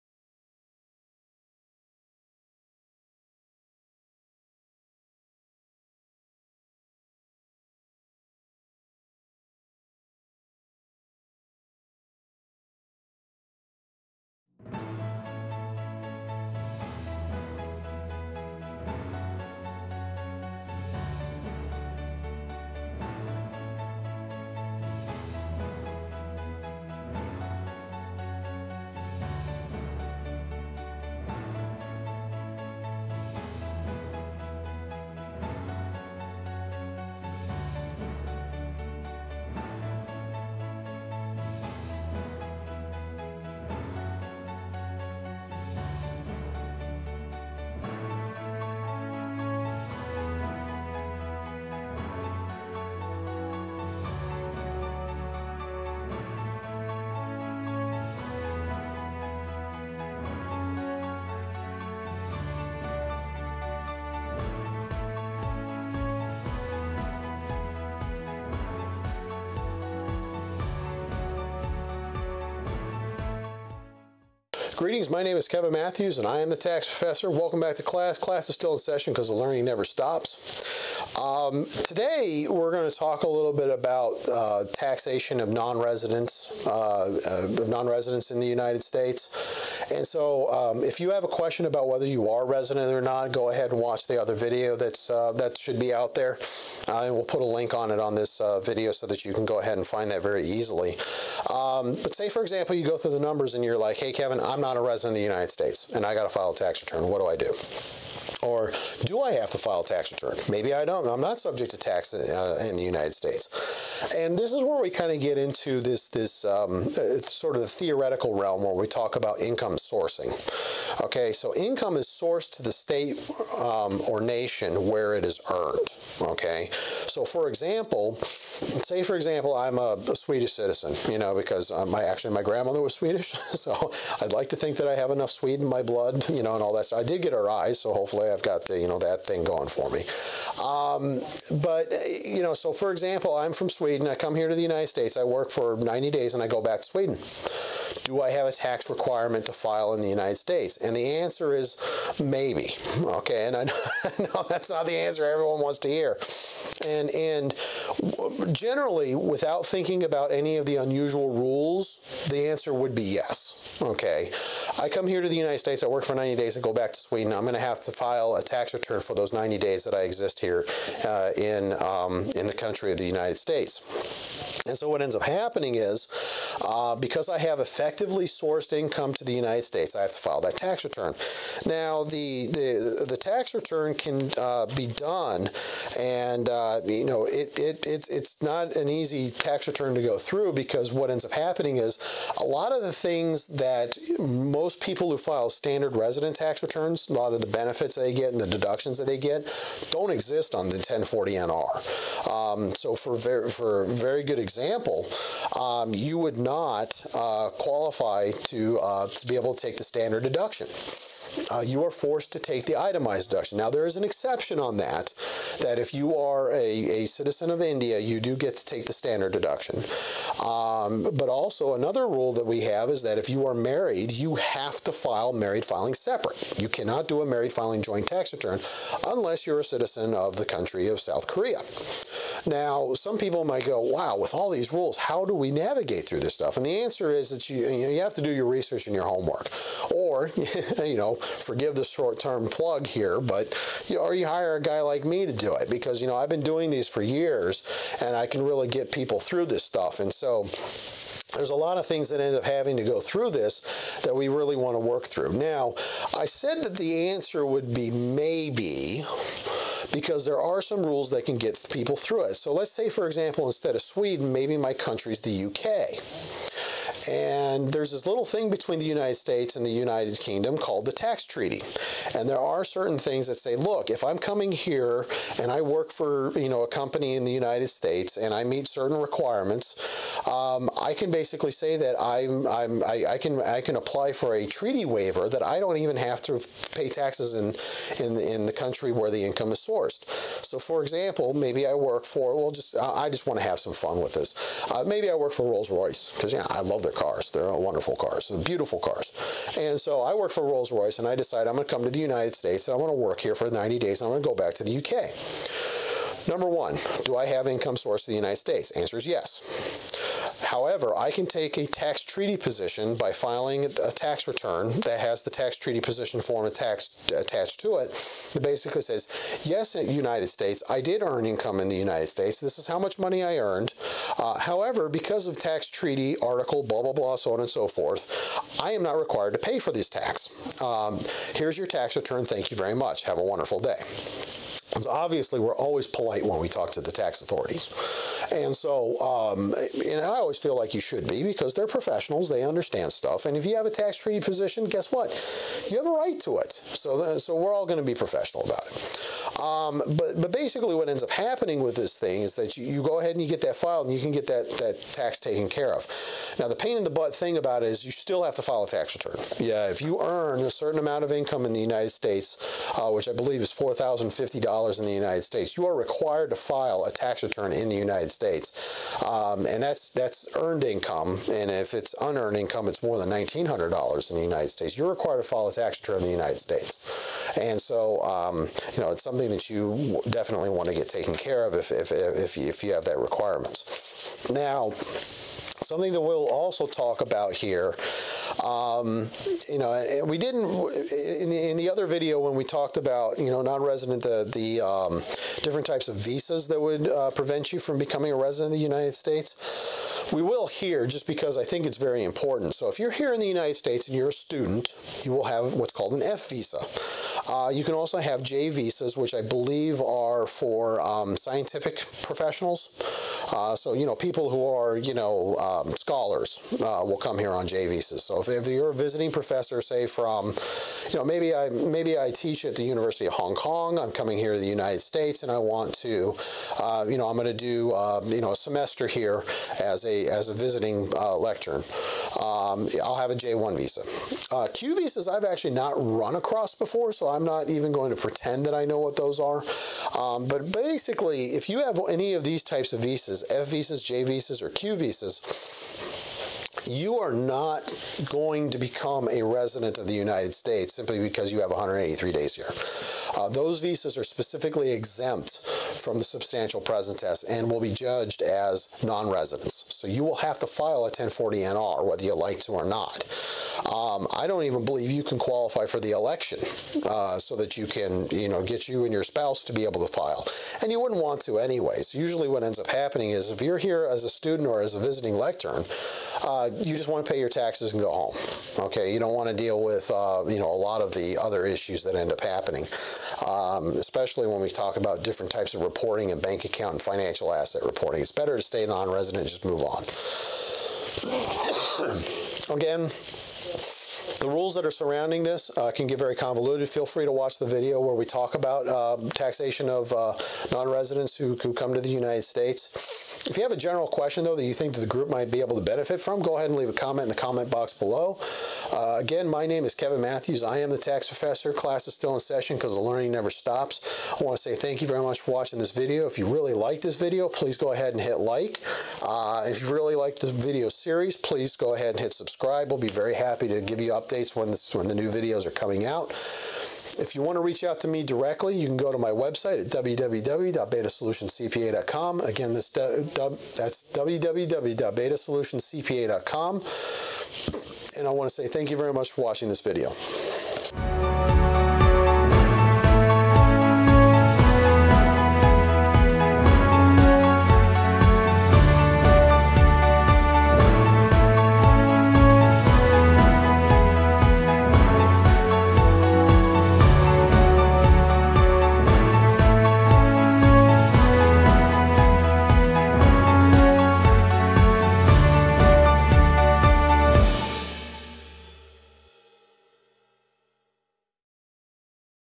Taxation of US Nonresidents - Beta Solutions CPA LLC
Are you a us non resident with income in the United States? Do you have investments in the United States, which are subject to tax?  Are you concerned that you might have a tax filing requirement in the United States?

In this video we discuss the filing requirements for tax returns in the United States if you are a US non resident.